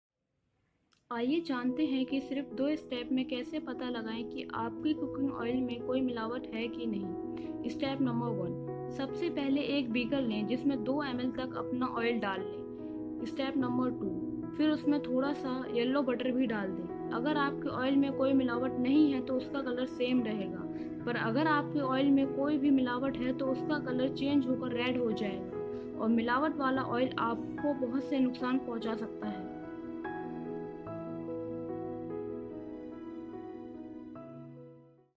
HOW TO CHECK YOUR COOKING OIL IS SAFE FOR YOUR HEALTH?
IN THIS VIDEO YOU WILL KNOW ABOUT WHETHER YOUR COOKING OIL IS SAFE FOR YOU OR NOT.